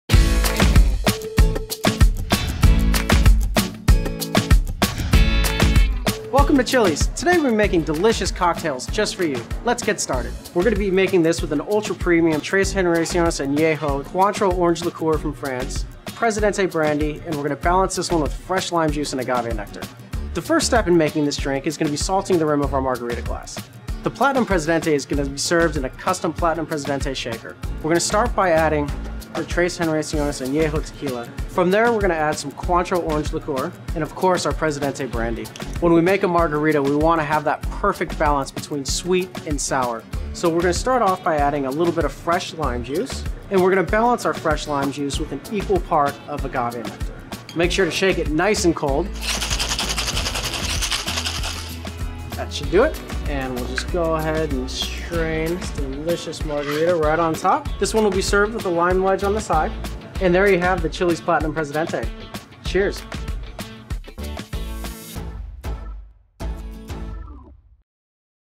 Chili's Platinum Presidente Short "How To" Reel
Chili's How-To Mix a Platinum Presidente Margarita